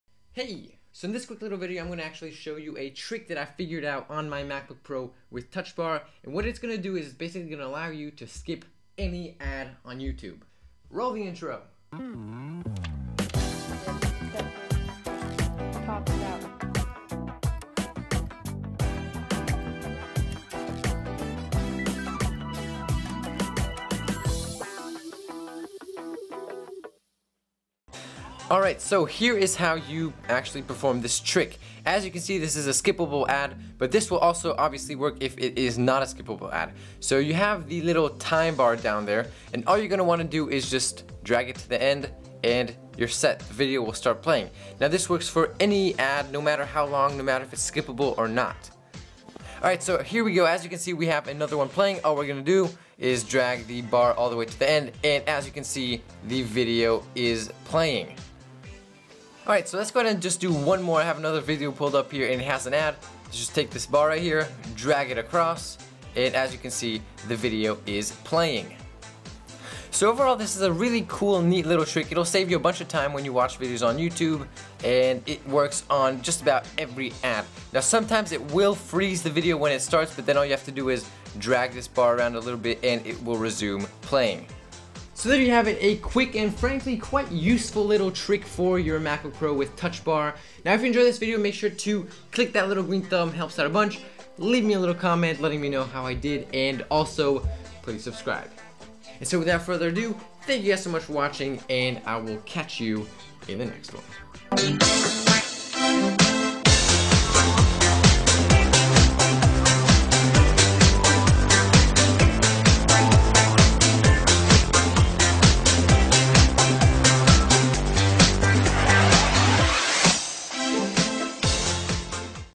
Touchbar trick 2017 - SKIP ADS INSTANTLY [MacBook Pro]
Hey everyone!

In this video I show you an awesome little touchbar trick that has saved me tons of time already.